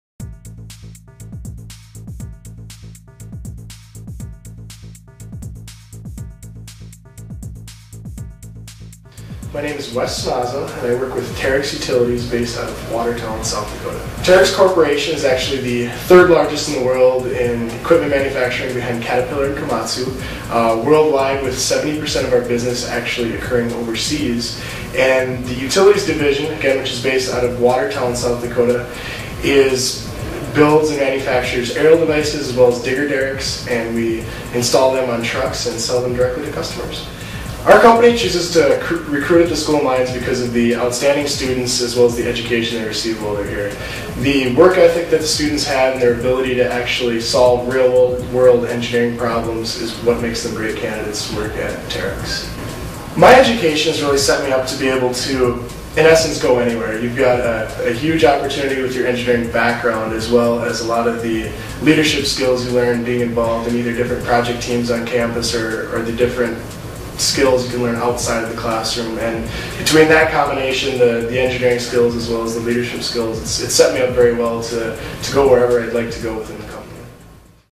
Terex Corporation Recruits South Dakota School of Mines and Technology Students
Terex Corporation testimonial regarding hiring South Dakota School of Mines and Technology students.